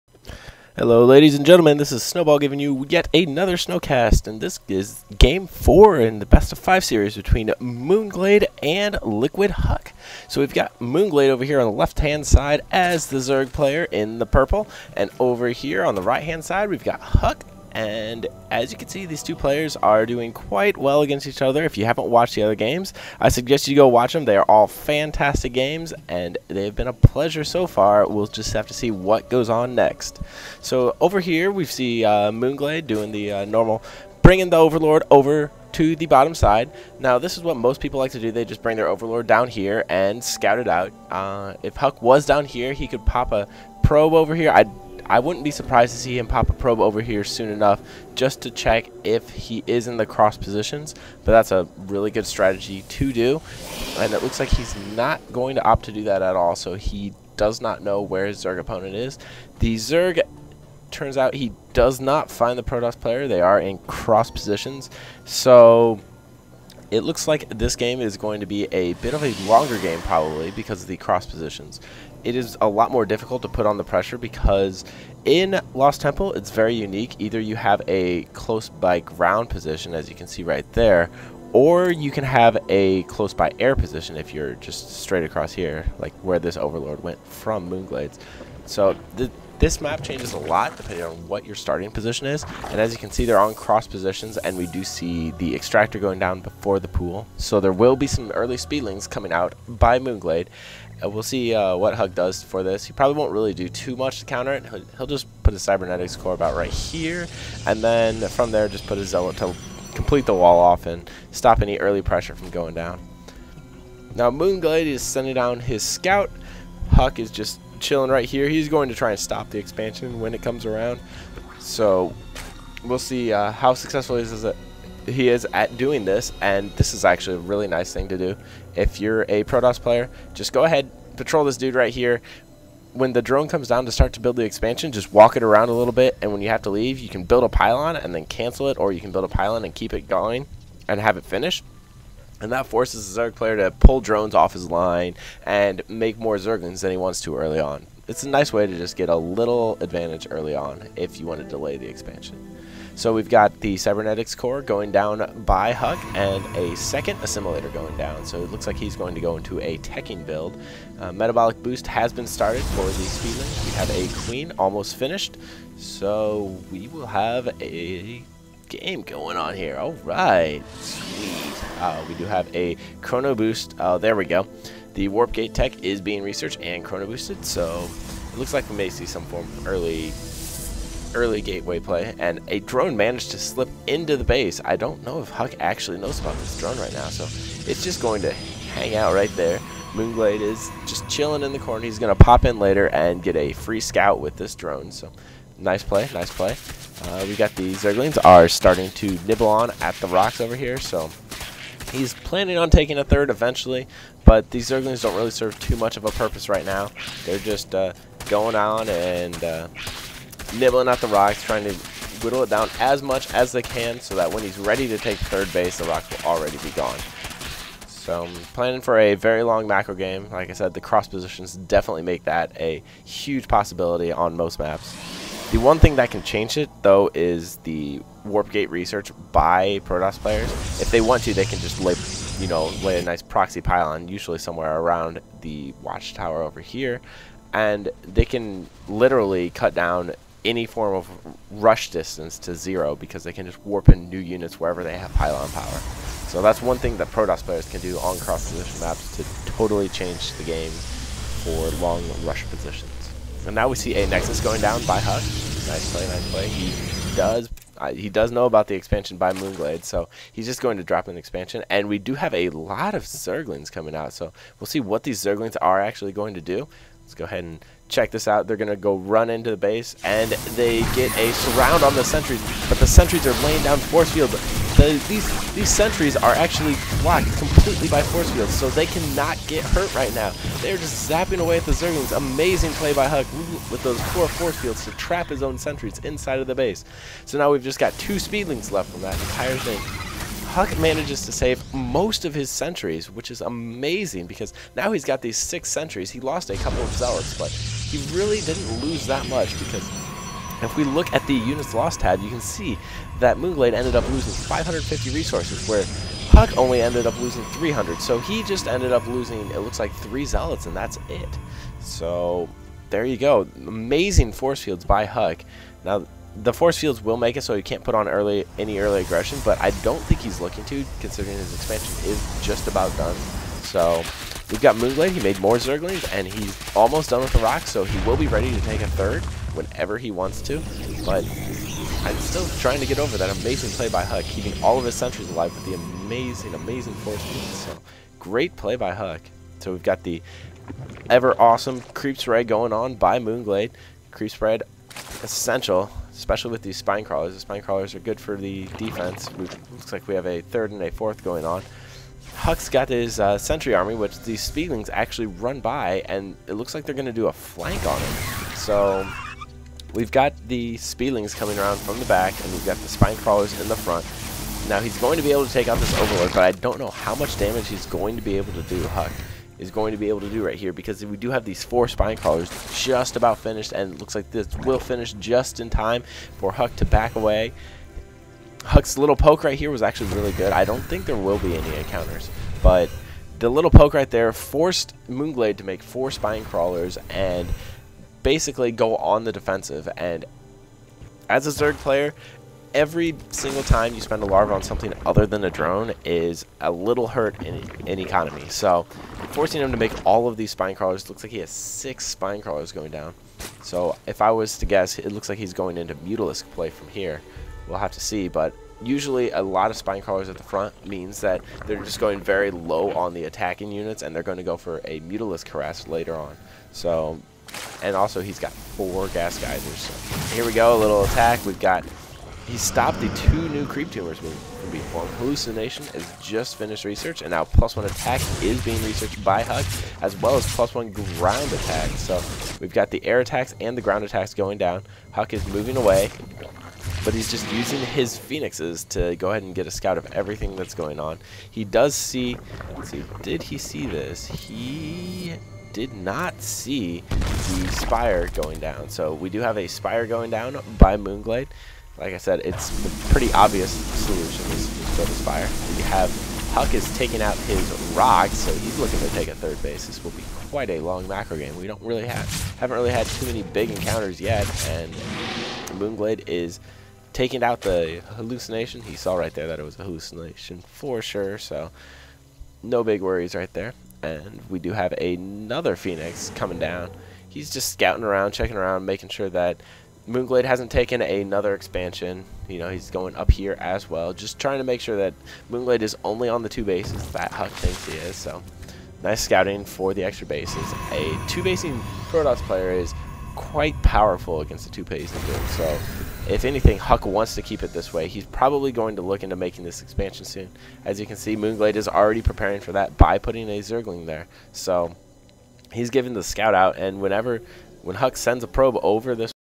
HuK vs Moonglade Game 4 Part 1
This is game 4 in a 5 game series against Huk and Moonglade. Enjoy!